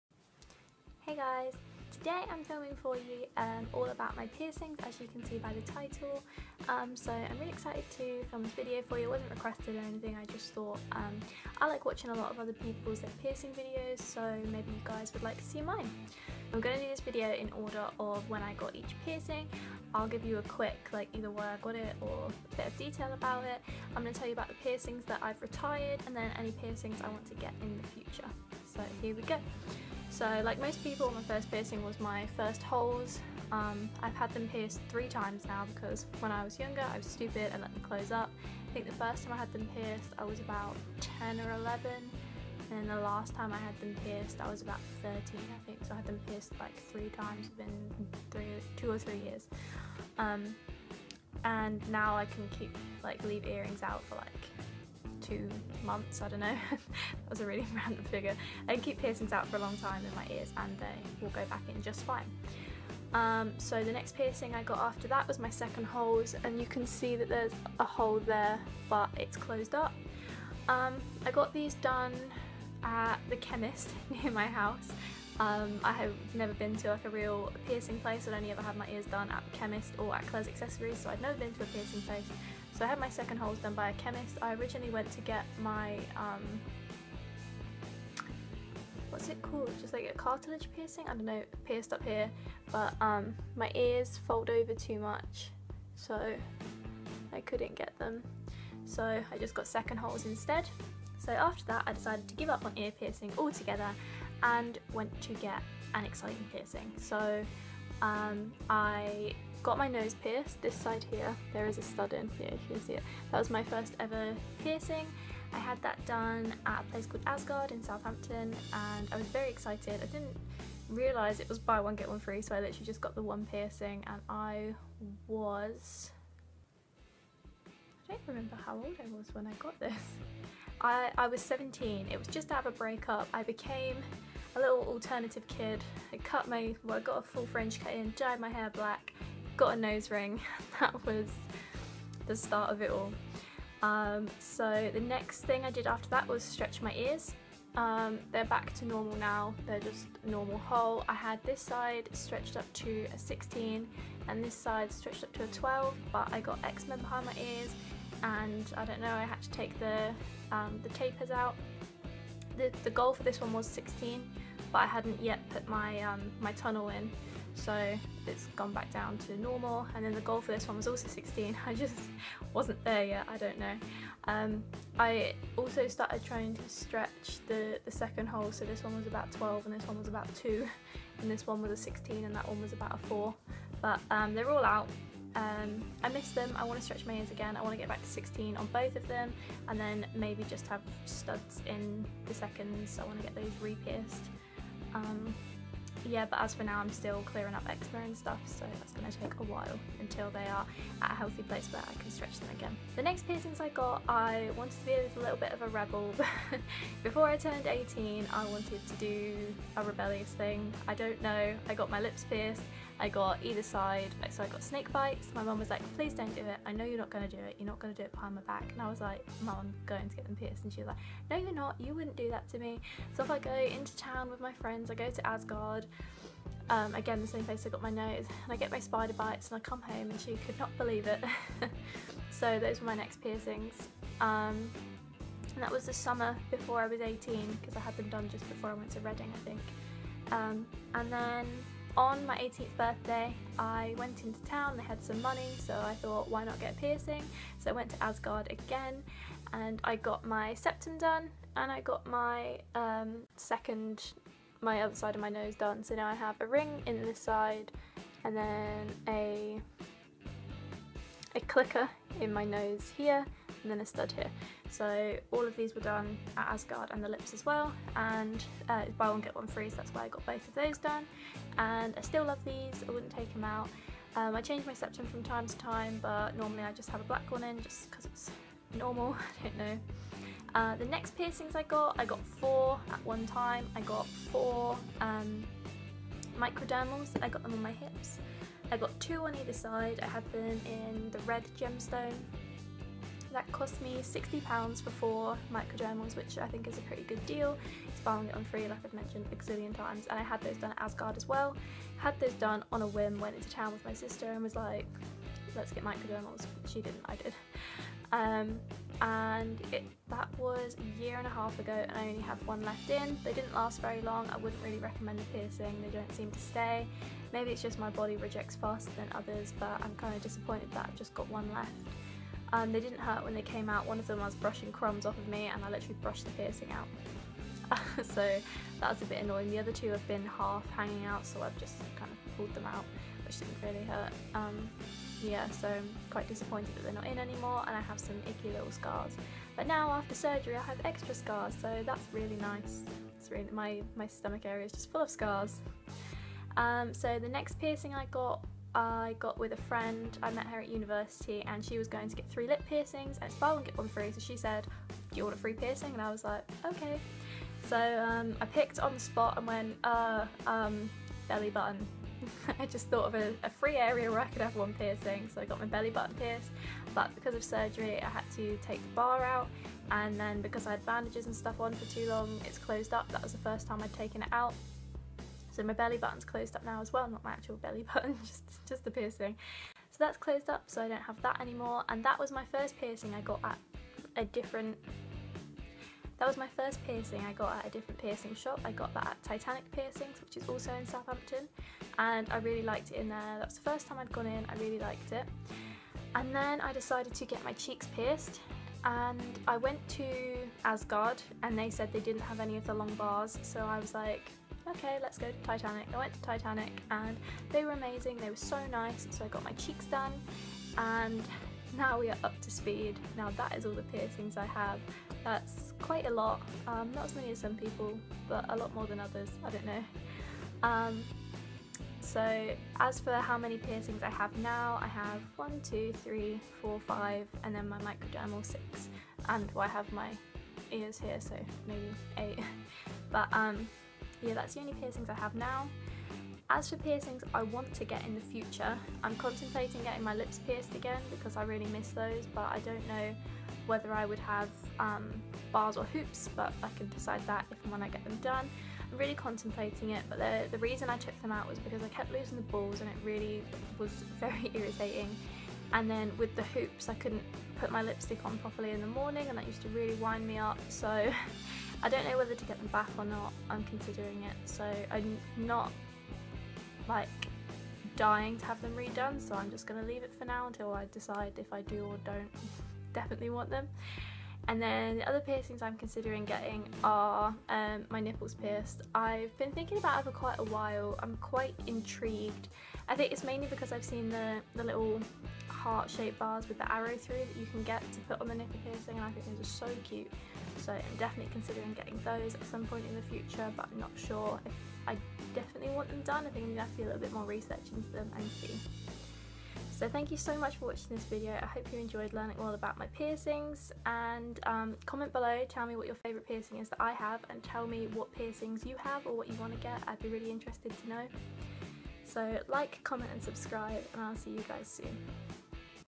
All About My Piercings
Here's a video all about the piercings I currently have, ones I have retired and my future plans for piercings! What piercings do you guys have? Let me know!

I love you all, my creatures of the night